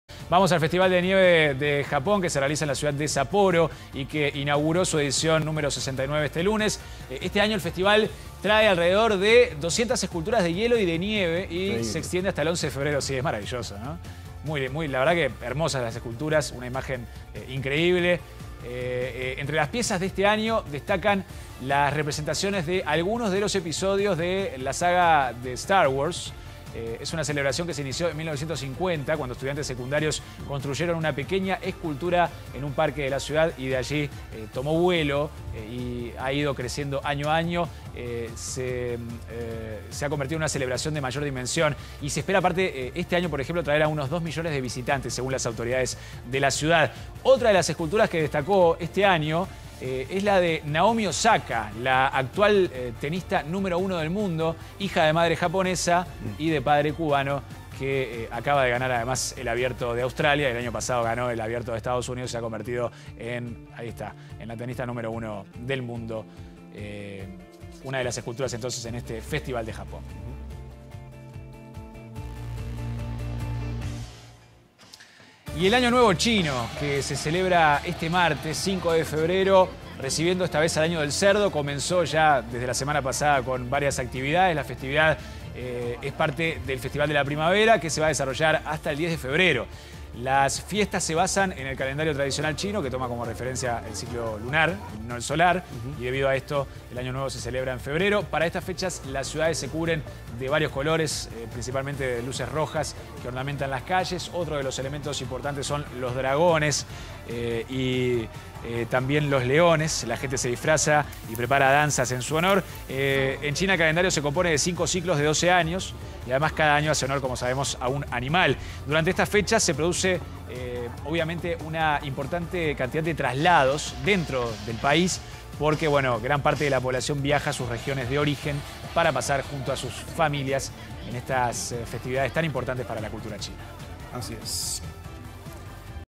Japón inauguró festival de esculturas de nieve y China le da la bienvenida al año del cerdo
Alrededor de 200 esculturas de hielo y nieve tiene el gran festival japonés y esperan atraer a cerca de dos millones de visitantes. Hasta el 10 de febrero durarán las celebraciones del festival de la primavera en China, donde se preparan para recibir el año del cerdo.